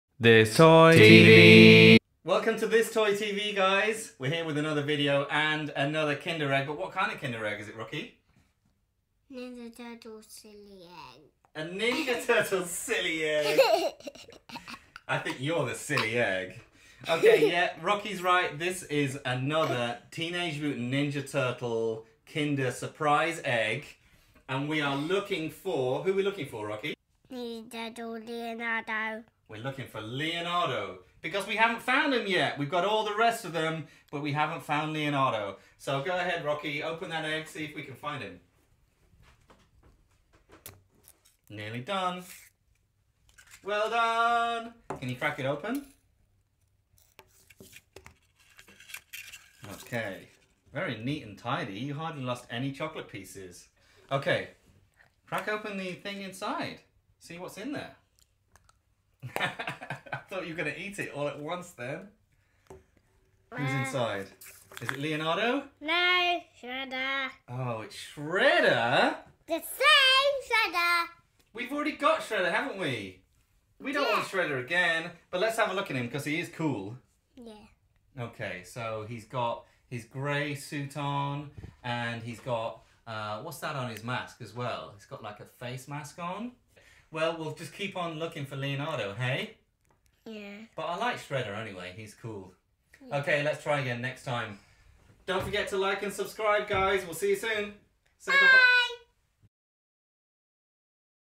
Kinder Surprise Egg Teenage Mutant Ninja Turtle Hunt Shredder
Rocky and his daddy hunt for Leonardo with Kinder Surprise Egg Teenage Mutant Ninja Turtles!